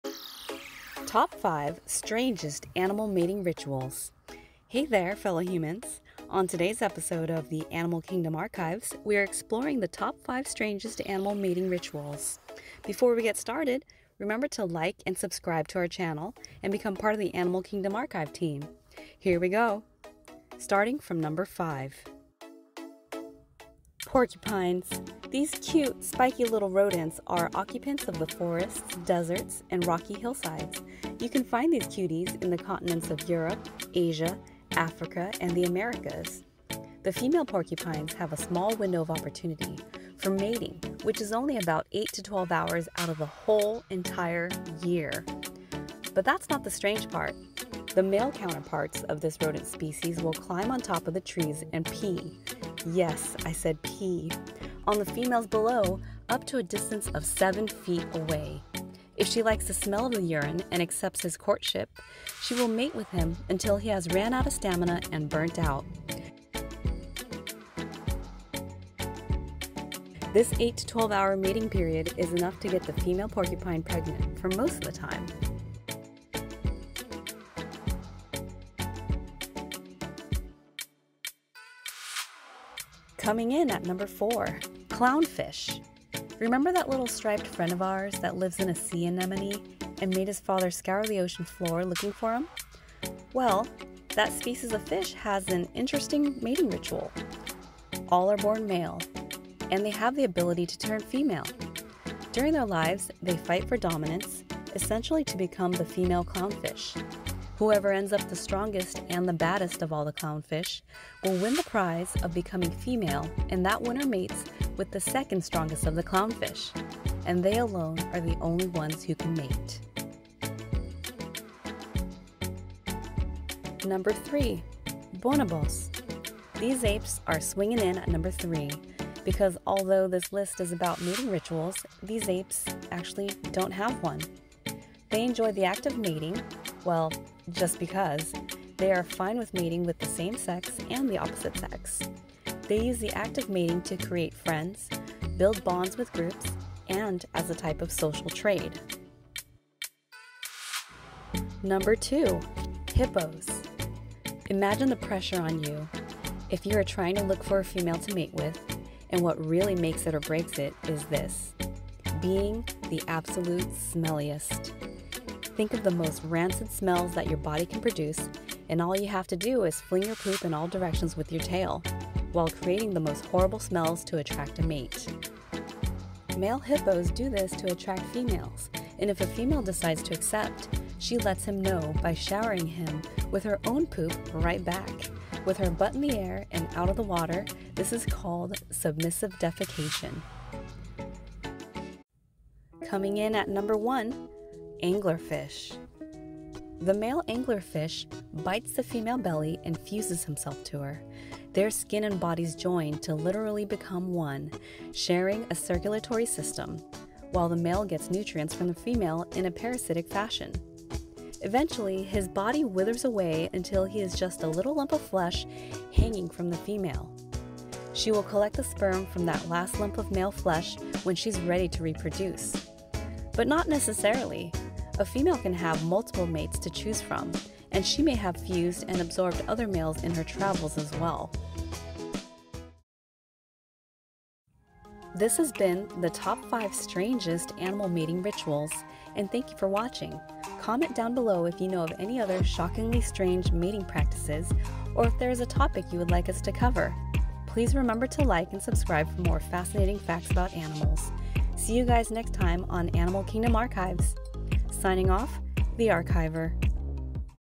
Top 5 Strangest Animal Mating Rituals
Welcome to Animal Kingdom Archives! This is a list of the top 5 strangest animal mating rituals. See if you can guess which one takes the top spot!